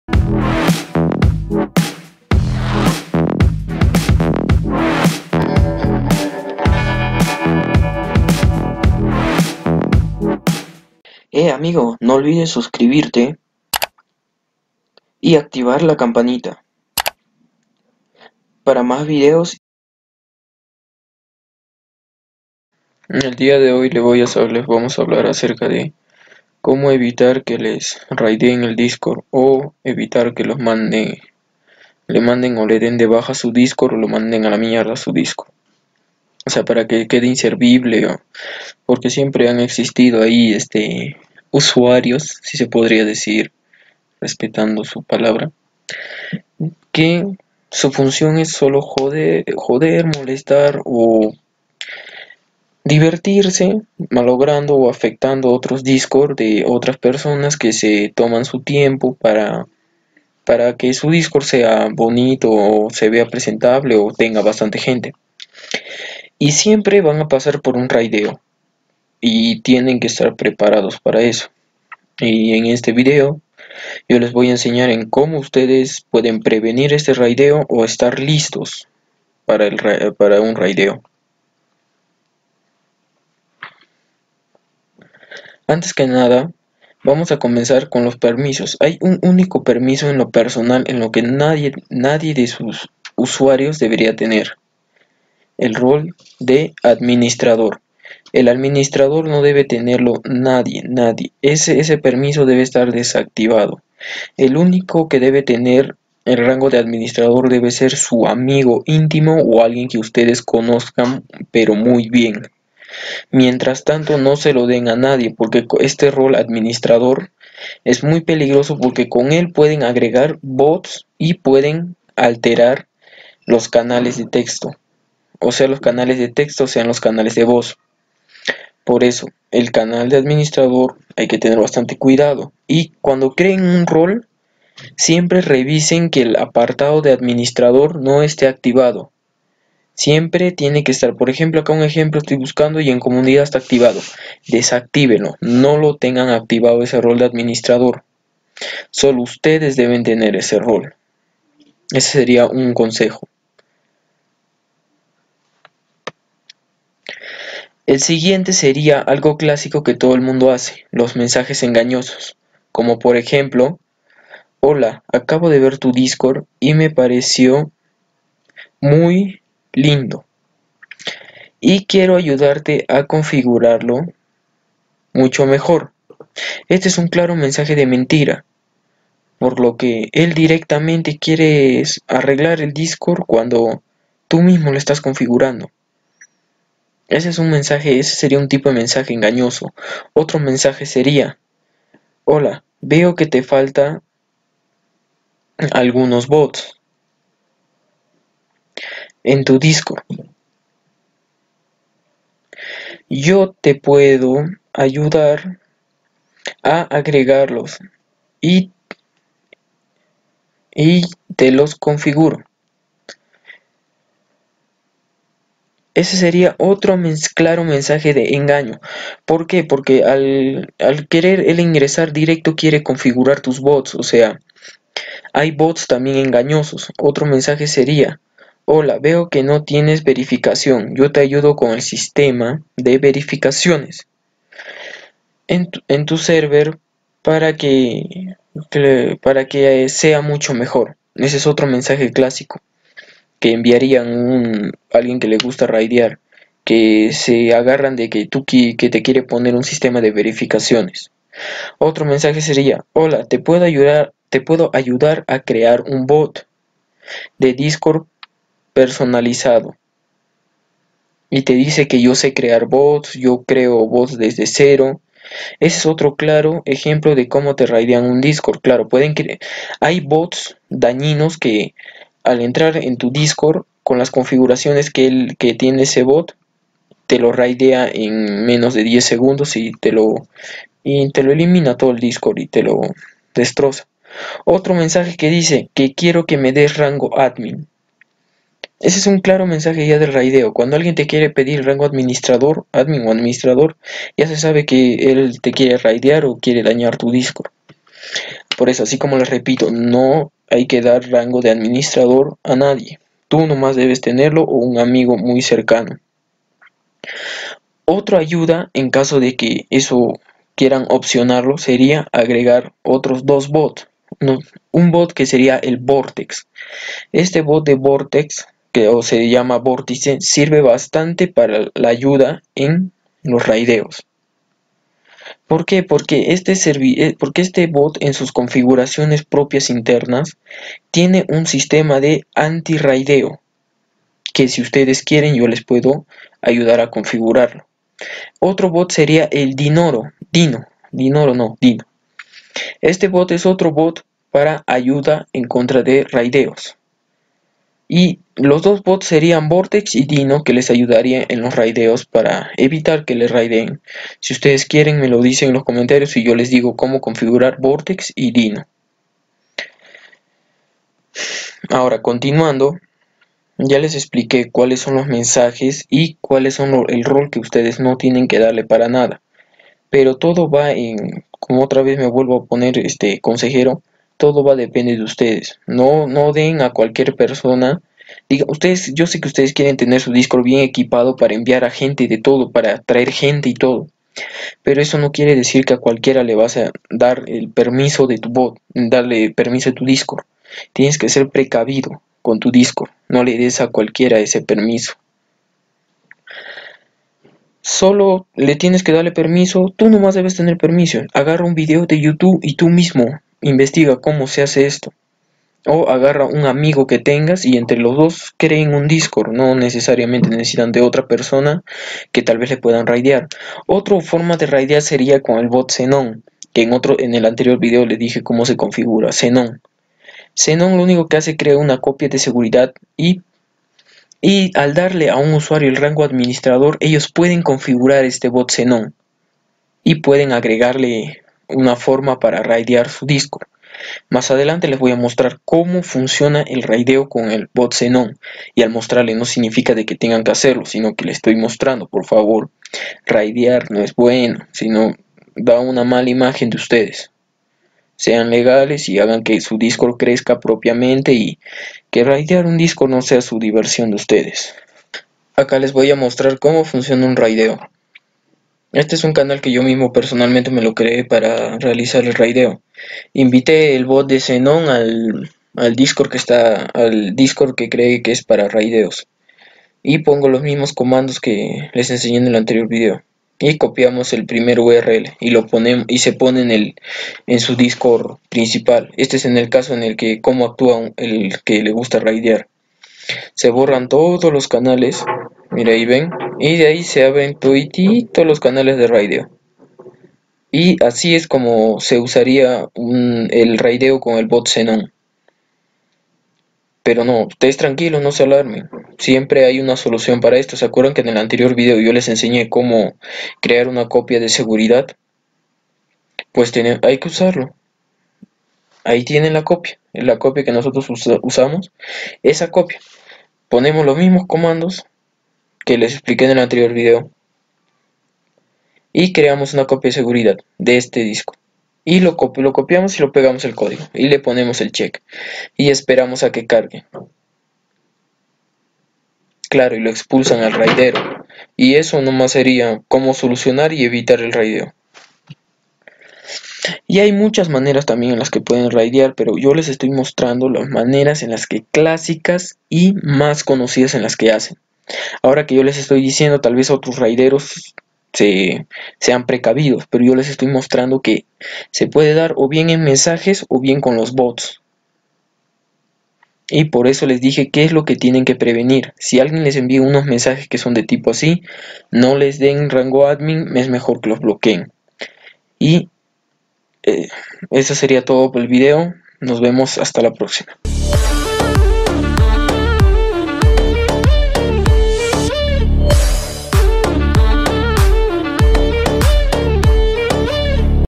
QUE HACER ANTE UN RAIDO EXPLICACIÓN Y SOLUCIÓN???? que hacer en un raideo
en este video les muestro como funciona un radio como afecta el discord y como podemos afrontar el raideo como reaccionar ante un raideo y que los que dañan su discord no se salgan con la suya

no olviden subcribirte para mas videos